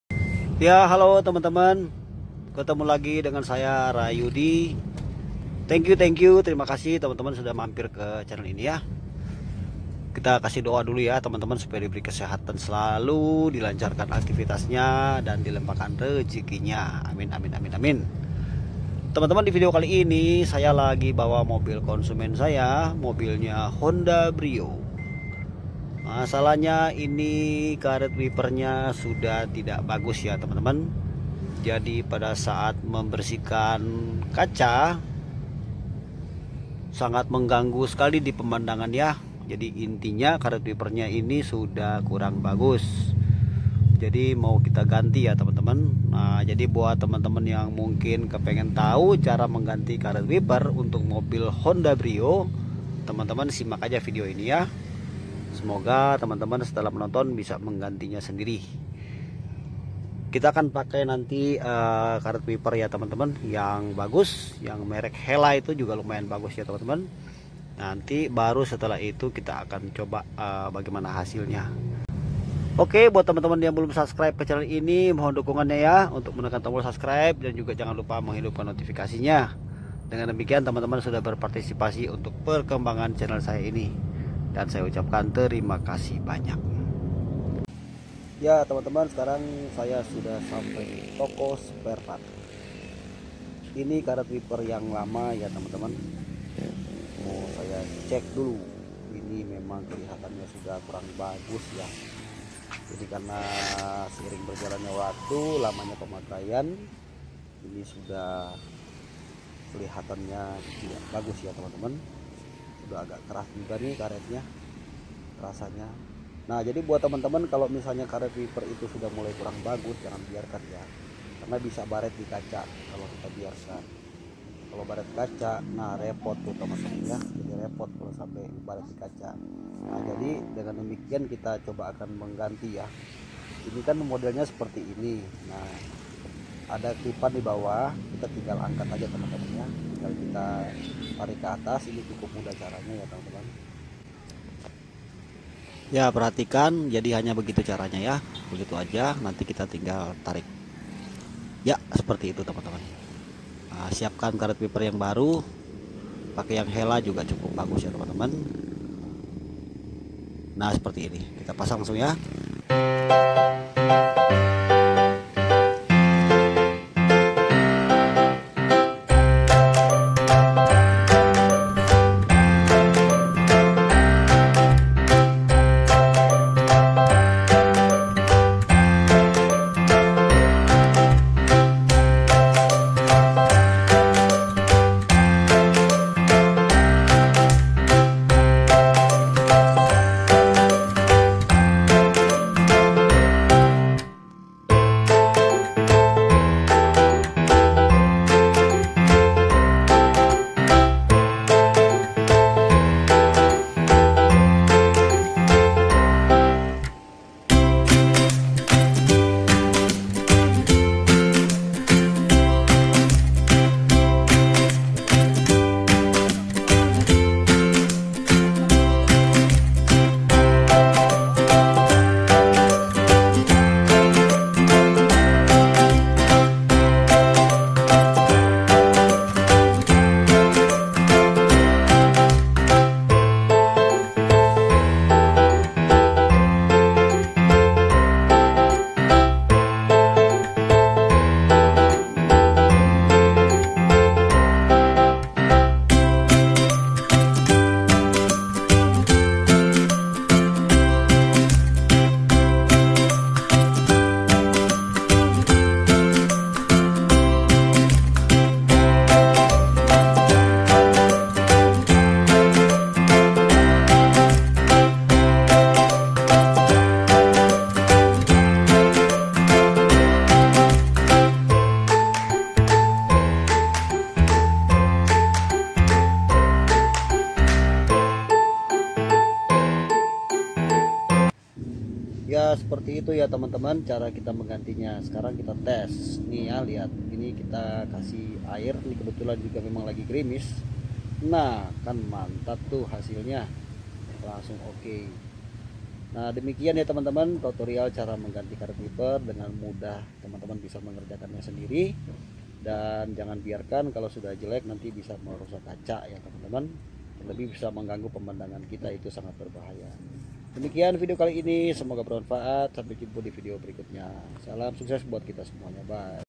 Cara mudah ganti karet wiper kaca depan Honda Brio
Tutorial cara mengganti karet wiper Honda Brio.
Semoga bermanfaat.